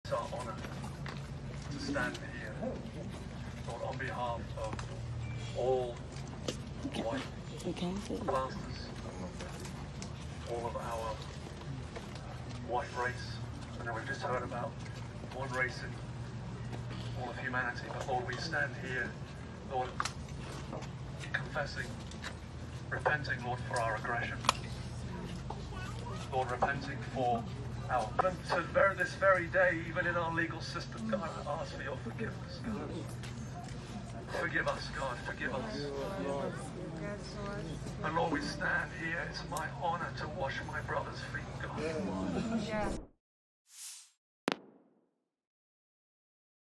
En Caroline du Nord, ces policiers prient avec des manifestants en mémoire de George Floyd
Dans la ville de Cory, en Caroline du Nord, des policiers se sont joints à des manifestants lors d'une prière à la mémoire de George Floyd, un Afro-Américain de 46 ans tué par un policier blanc lors d'un contrôle le 25 mai dernier à Minneapolis. Ils ont posé un genou au sol et ont lavé les pieds du pasteur qui officiait. Dans la Bible, ce geste est considéré comme un geste d'humilité.